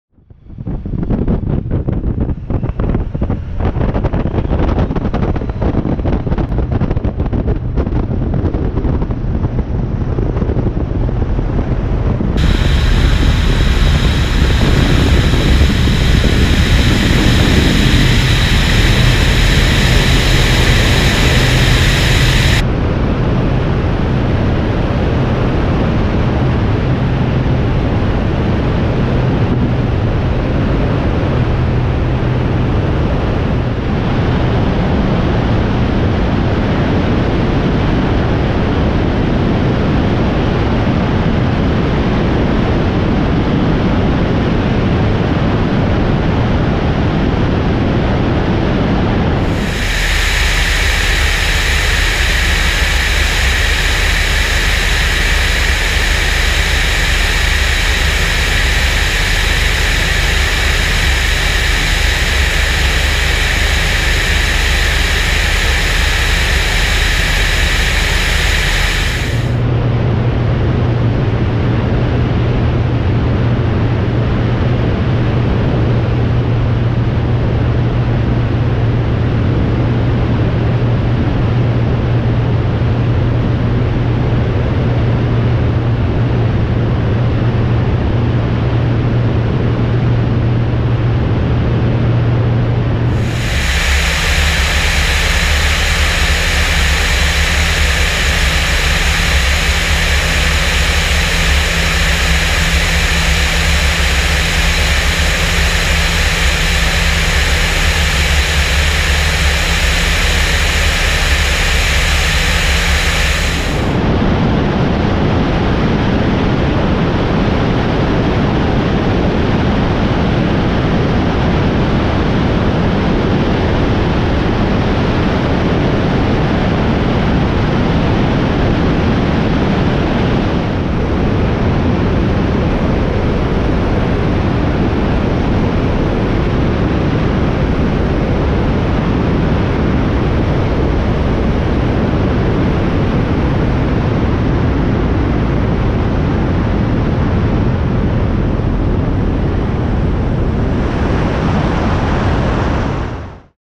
It's always sunny above the clouds
Europa flight to the East of Fife airfield, Scotland